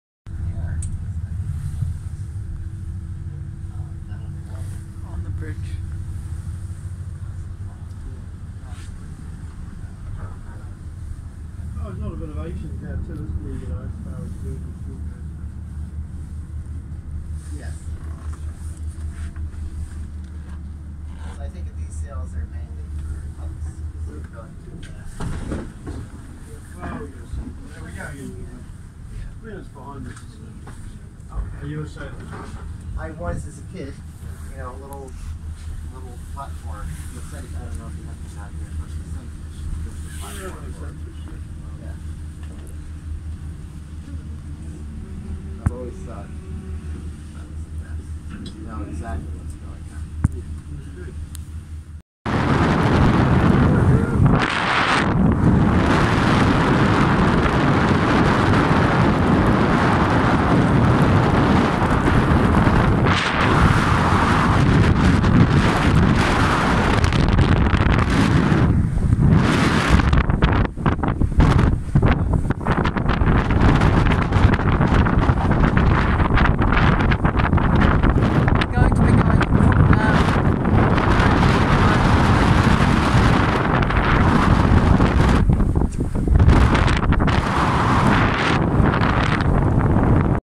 Sailing on the Real Journeys ship in Doubtful Sound / Pātea, New Zealand
I wander through the bridge and onto the foredeck of the Real Journeys overnight cruise ship as it makes its way up Doubtful Sound / Pātea, in south-west New Zealand, on 19 September 2019. This is in one of my blog posts.